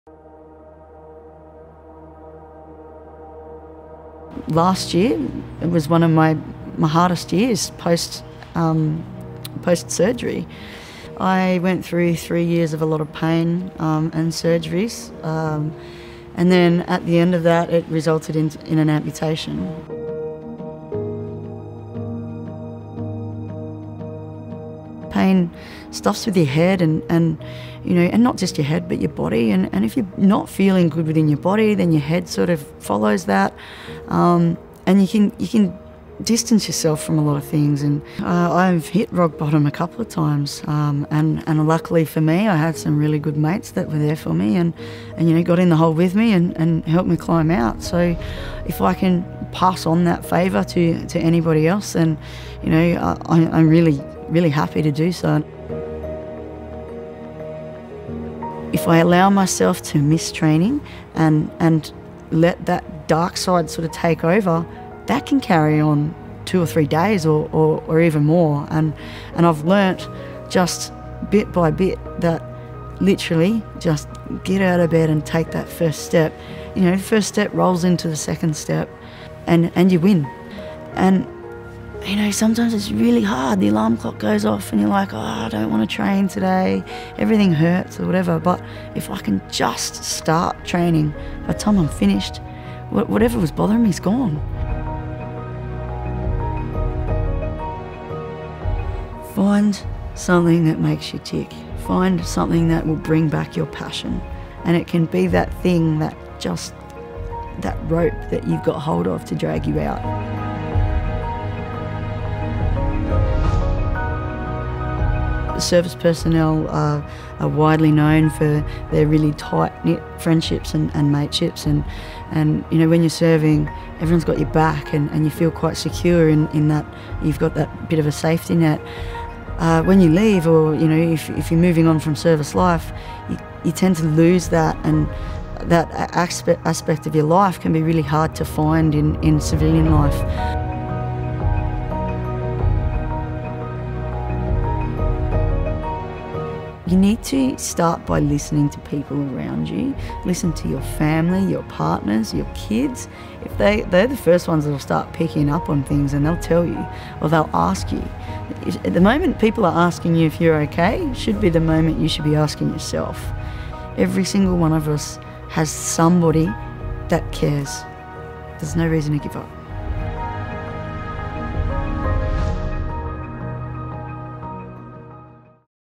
Sonya shares her Invictus Games story
Invictus Games’ competitor, Sonya shares her journey to the 2018 Invictus Games. Beyond Blue and Invictus Games Sydney 2018 are working together to highlight the positive impact that sport, physical exercise and participation can have on mental health and wellbeing.